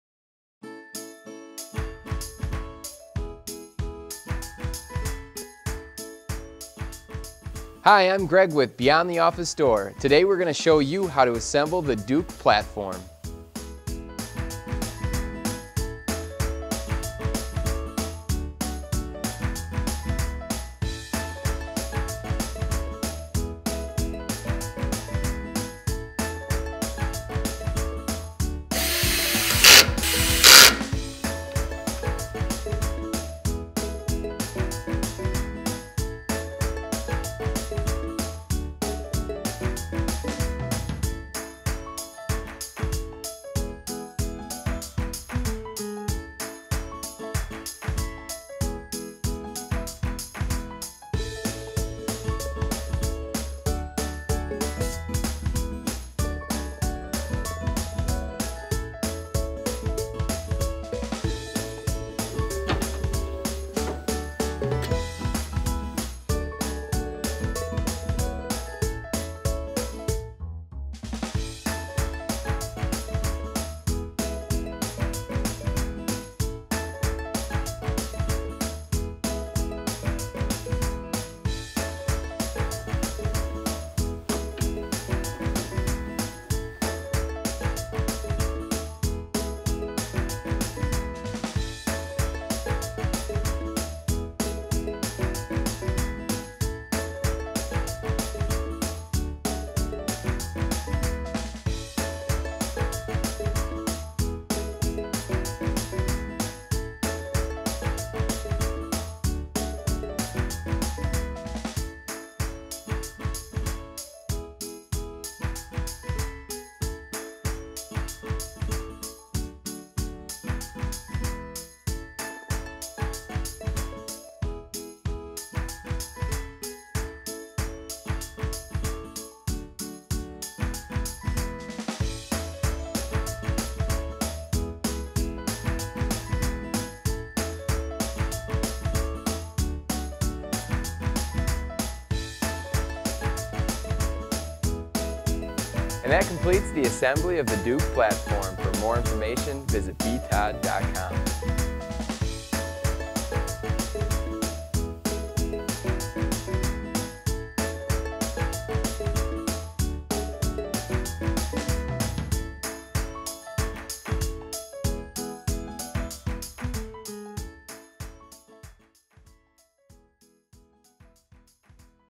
Assembly - The Duke Single Platform Standing Desk Converter
Easy to follow video to help you get started assembling your new Duke Platform.

The Duke platform system is perfect if you are using monitors without VESA compliant mounting systems or laptops of various sizes. Just set your existing monitor or laptop on the platform and you're ready to stand.